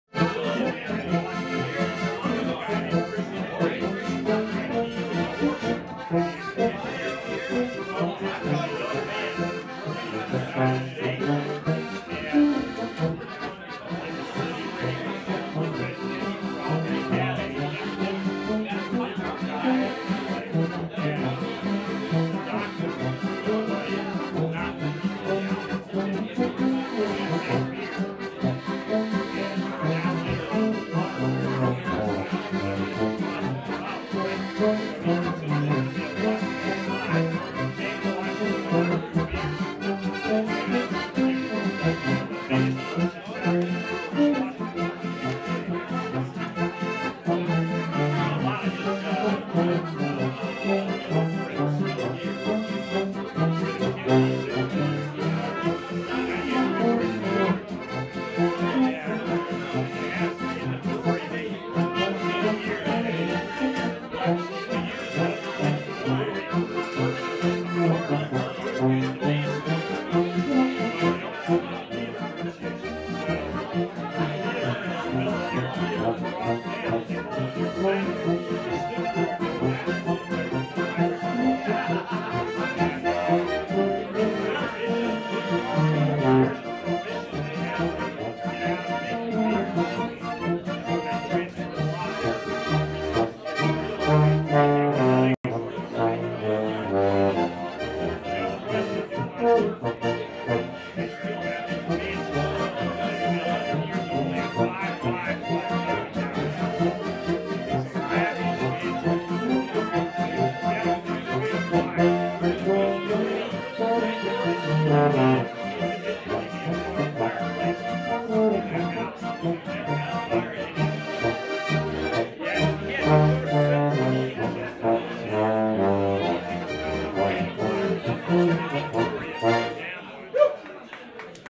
Musical Cheer at the Old Europe Restaurant
Michael Jr, Elisabeth, and Mark provide some Volksmusik at the Old Europe Restaurant in Washington DC.  It was a great time had by all celebrating the upcoming holidays!